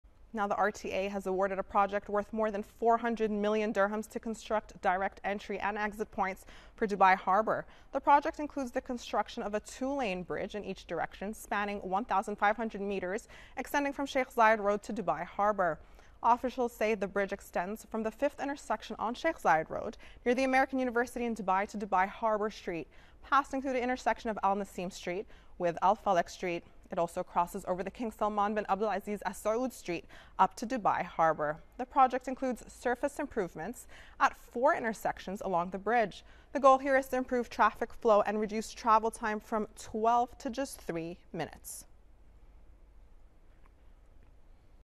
RTA awards a project to construct direct entry and exit points for Dubai Harbour
Emirates News